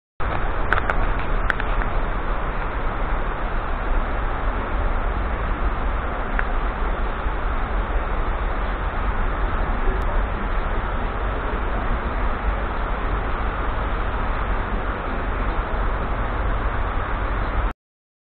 Тихий час у голубей. Как спят голуби
На улице дождь , и птицы безмятежно спят под навесом. Если кто то не видел как спят голуби это происходит так, да как у куриц. А заметили что один глаз у голубя на стрёме?